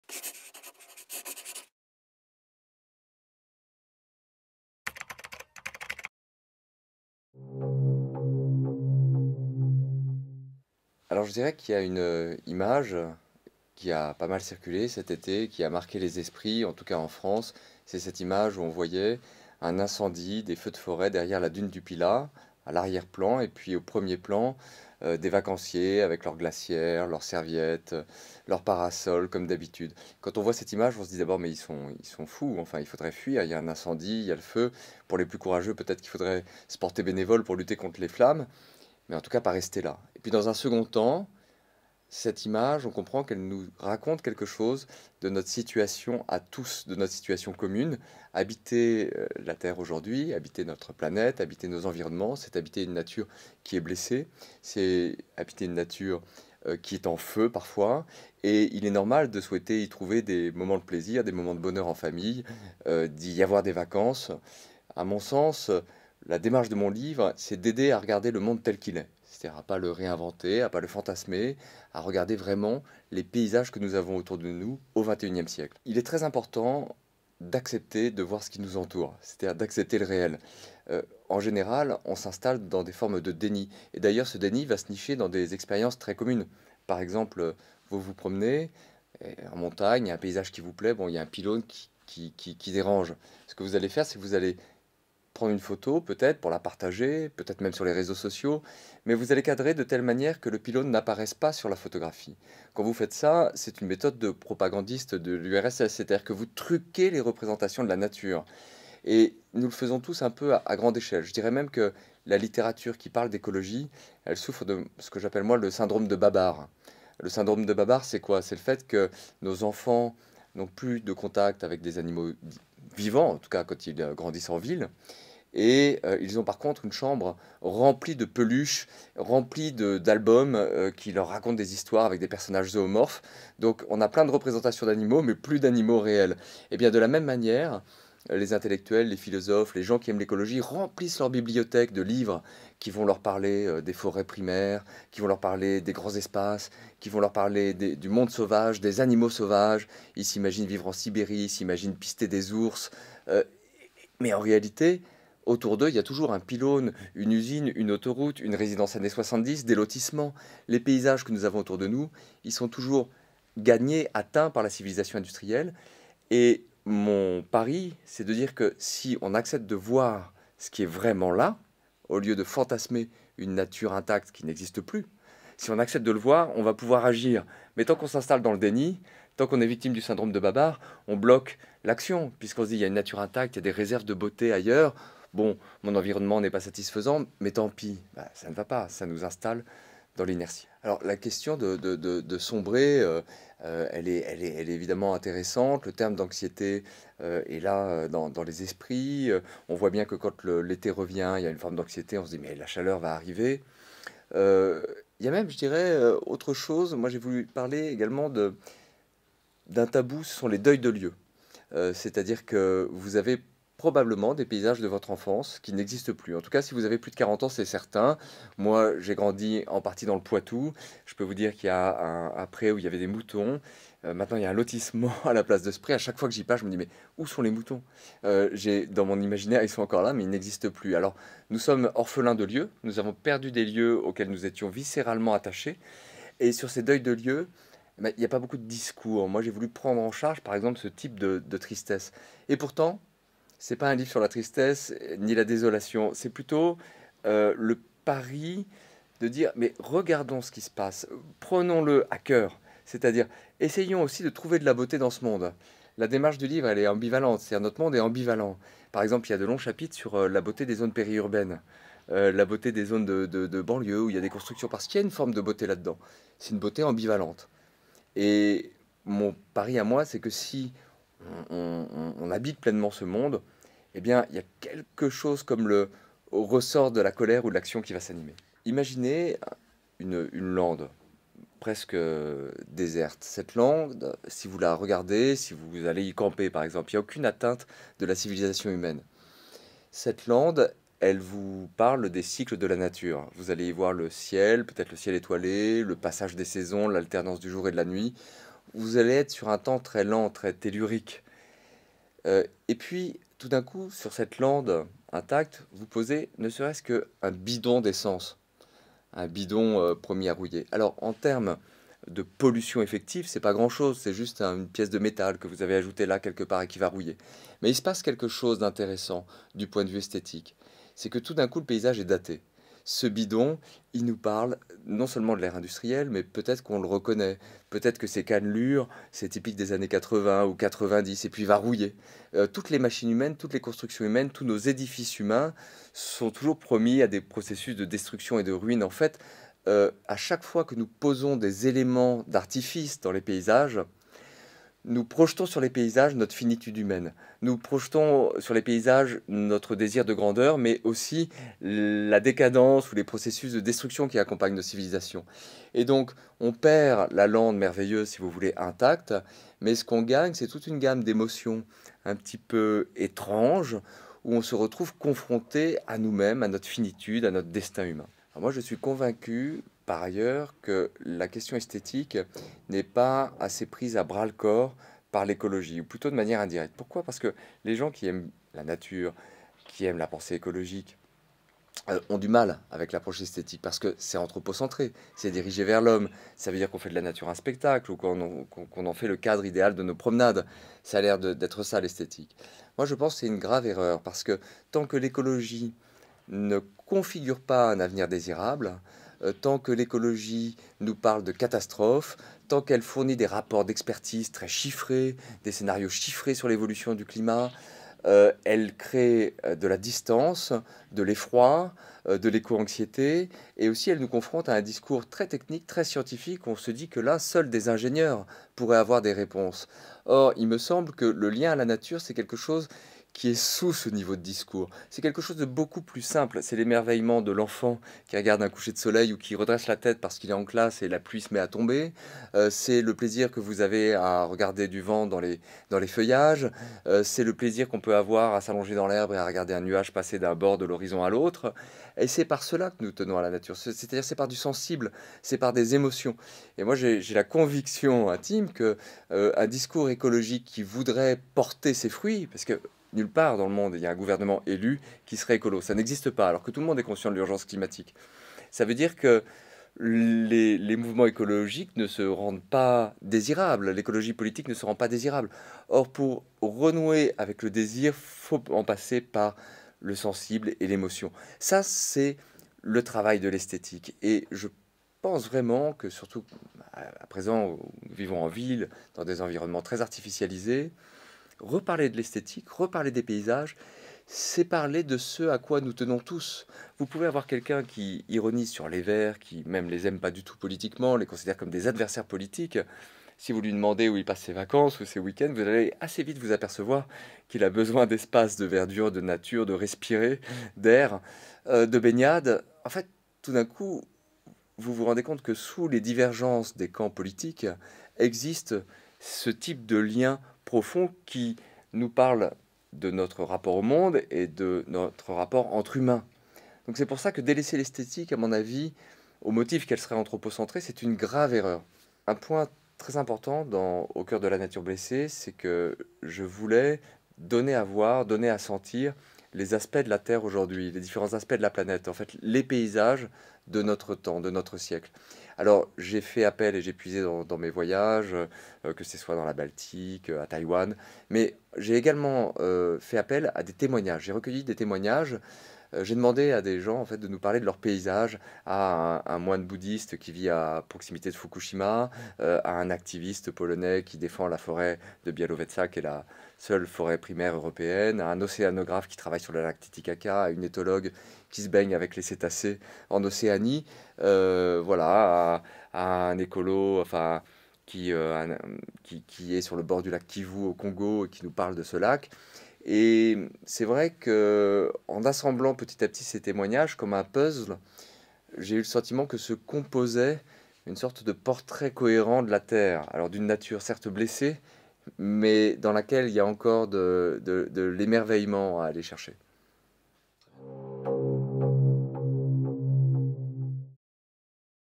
Alexandre Lacroix - Au coeur de la nature blessée : apprendre à voir les paysages du XXIe siècle
Alexandre Lacroix vous présente son ouvrage "Au coeur de la nature blessée : apprendre à voir les paysages du XXIe siècle" aux éditions Allary.